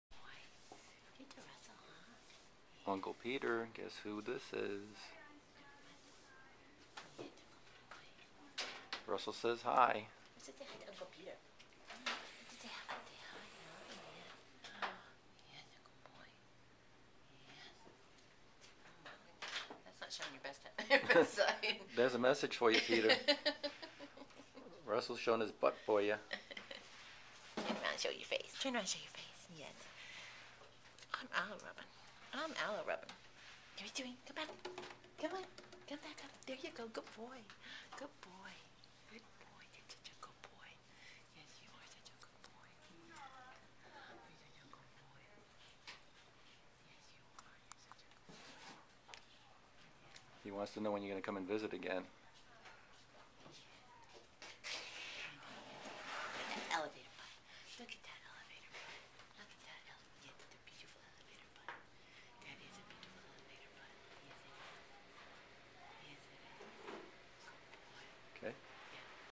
Russell A GKR Cat for Adoption
Russell, a survivor of the Great Kitty Rescue lives in Room 3 at Jill's Diner at Best Friends Animal Sanctuary, while he waits for his forever home. He longs for the day when Peter Wolf comes back to visit him...